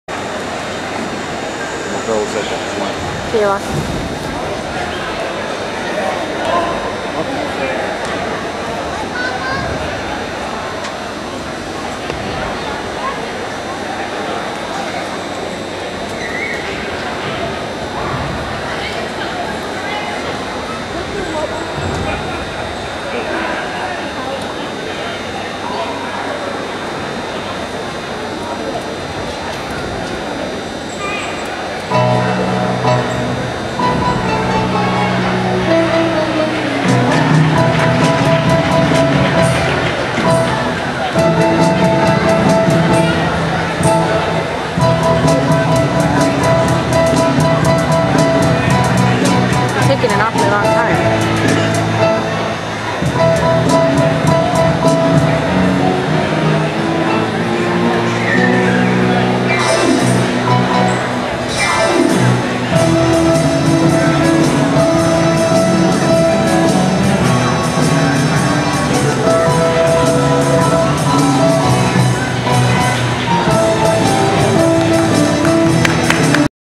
Illinois Judge's Cup - Uneven Bars
Kristina received a 8.90. She placed 1st in this event. She placed 1st all around at this competition.